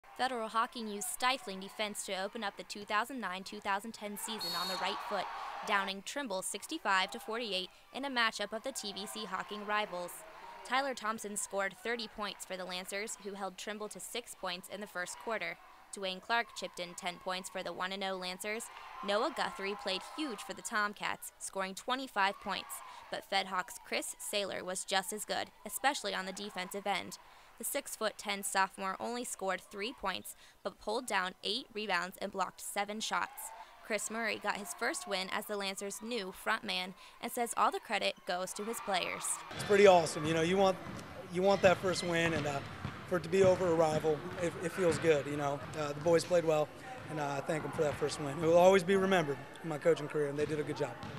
Trimble at Federal Hocking Recap
The Federal Hocking Lancers opened their season with a 65-48 win over conference rival Trimble and Hardwood Heroes' reporter Kristen Walters was there to cover the action.